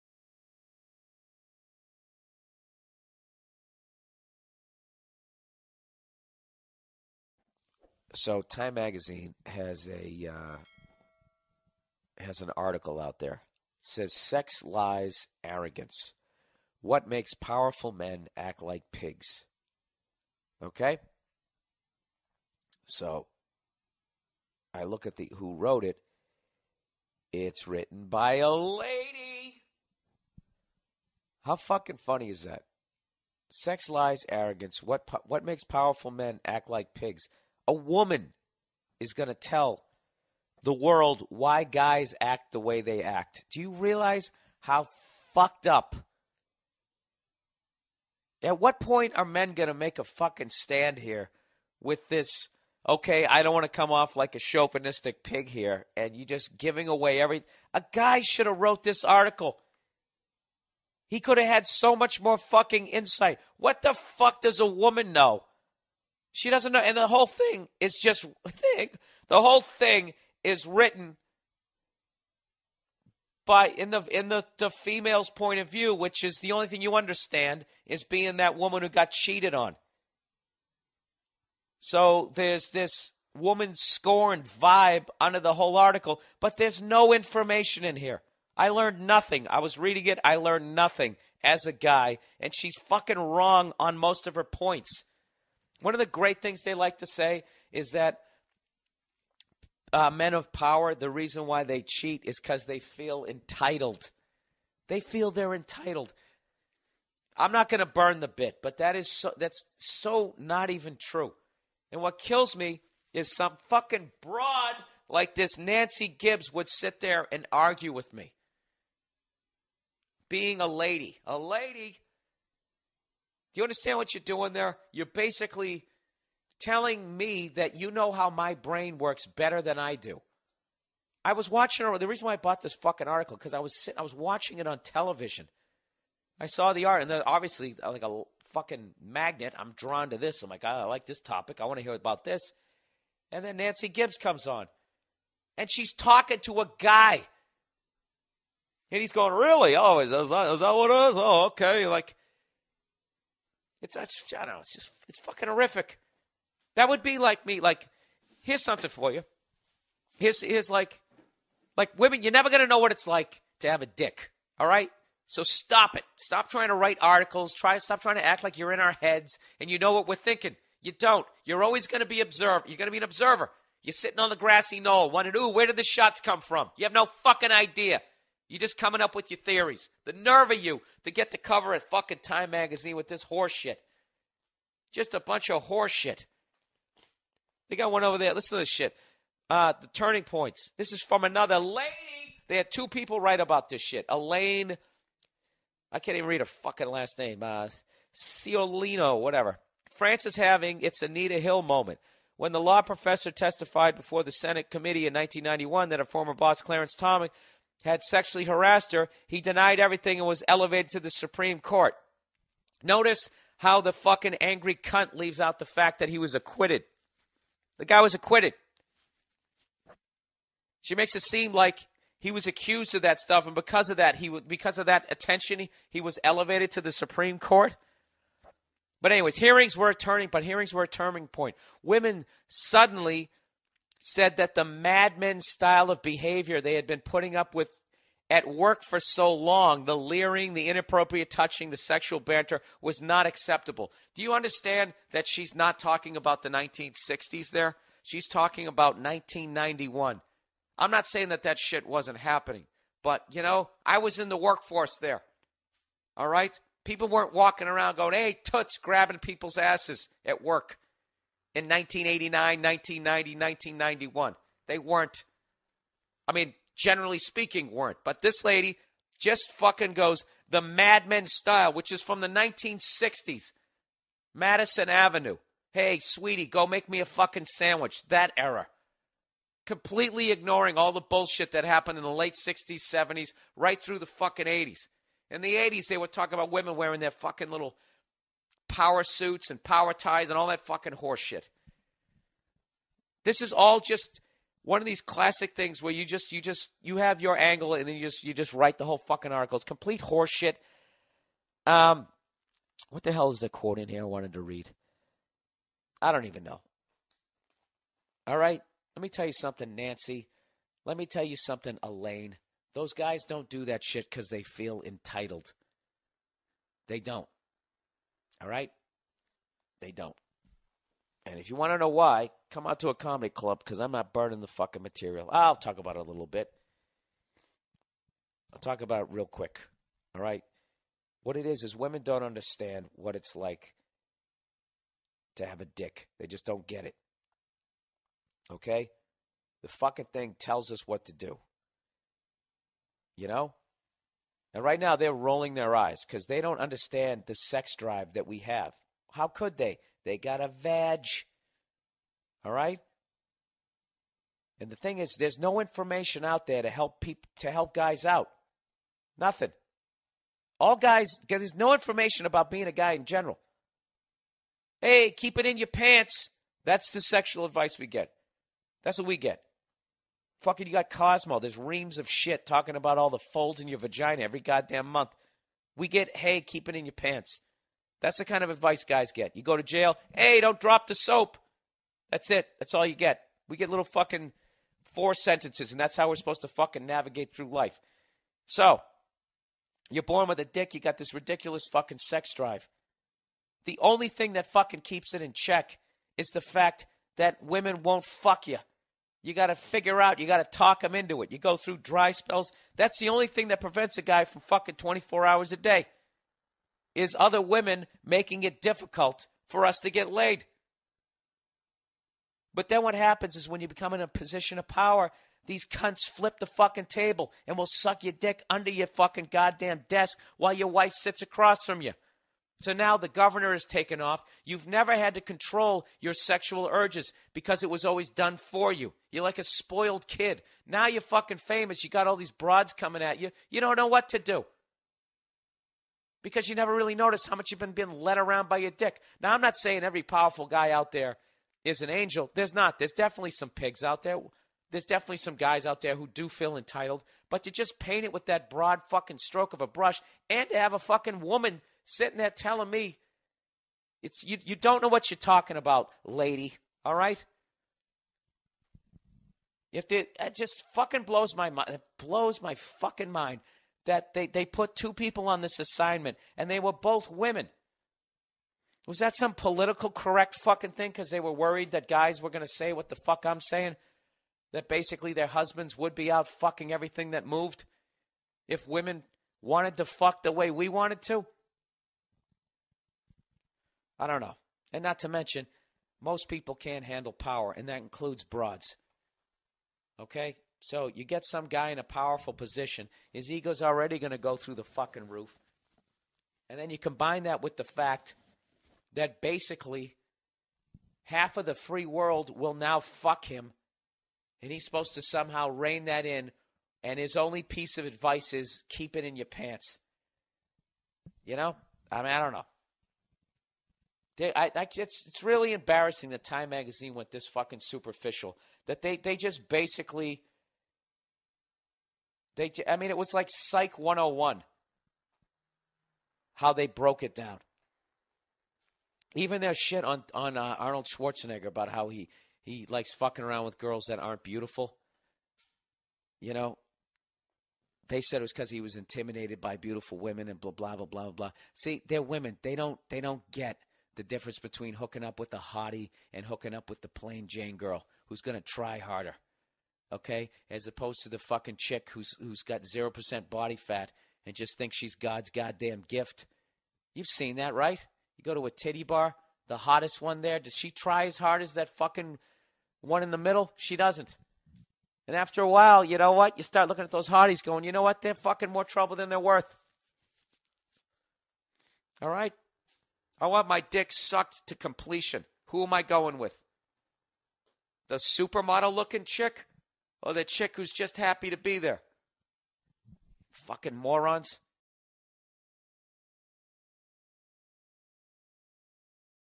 Bill Burr - Why Men in Power Get More Sex
Bill Burr goes off on how womansplaining is ruining society and why men in power aren't pigs when life offers them free sex.

Bill Burr is an American stand-up comedian, writer, actor, and podcaster who has released several stand-up comedy specials. Outside of stand-up comedy, he is known for hosting the Monday Morning Podcast, playing Patrick Kuby in the AMC crime drama series Breaking Bad, and creating and starring in the Netflix animated sitcom F Is for Family.

Bill burr is the lord.